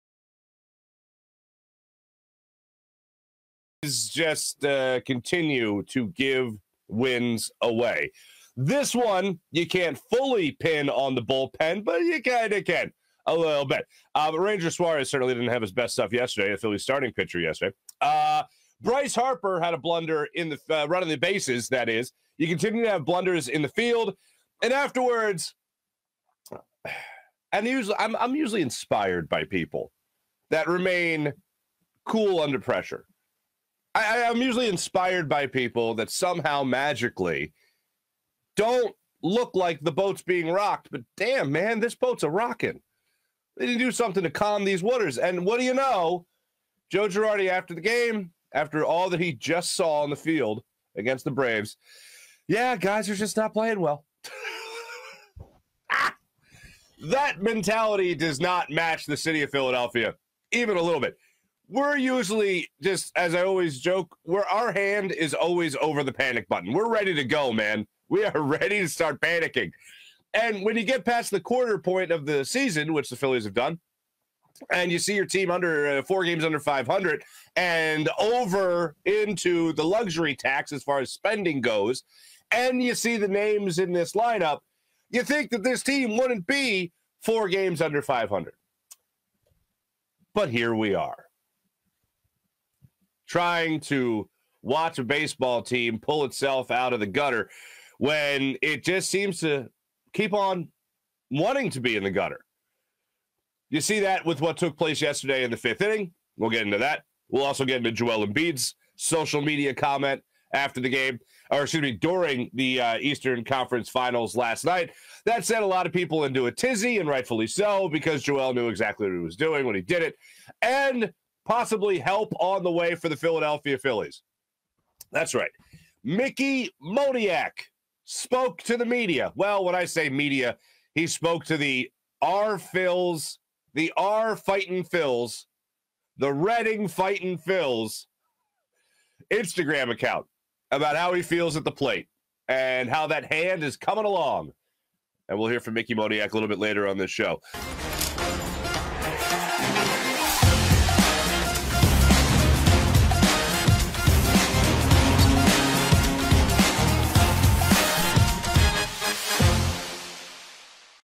Phillies GIVING away wins | Joe Girardi content with Phillies play?! | Mickey Moniak to the rescue?
Phillies lose another one to the Atlanta Braves and Marc Farzetta reacts to the Phillies manager post game comments about the team "not playing well right now". Is it finally time to hit the panic button on this team?

- Destination Retirement
- Steven Singer Jewelers
- Rothman Orthopaedics
- Sky Motor Cars
- PHLSportsNation.com